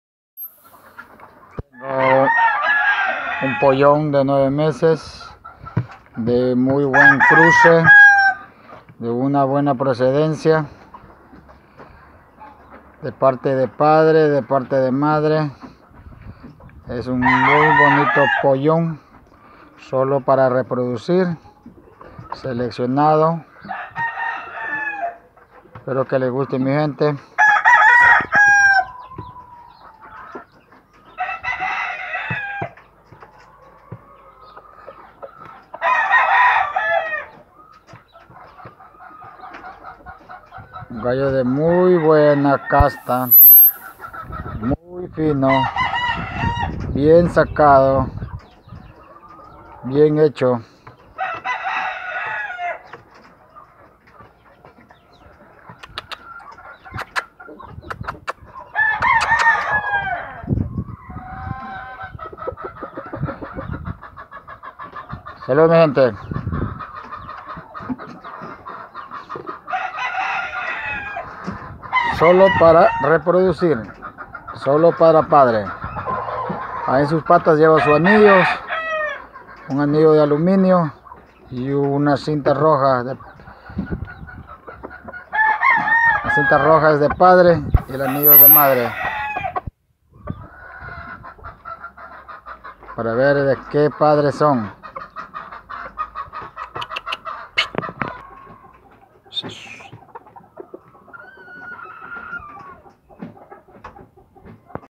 GAMEFARM EL CABO POLLON MUY FINO SOLO PARA APARIAR PARA SEMENTAL
DUEÑO JUAN CARCELEN EL CABO POLLON SOLO PARA ENRRAZAR PARA SEMENTAL PARA APARIAR FINO FINO POR DONDE LO VEAN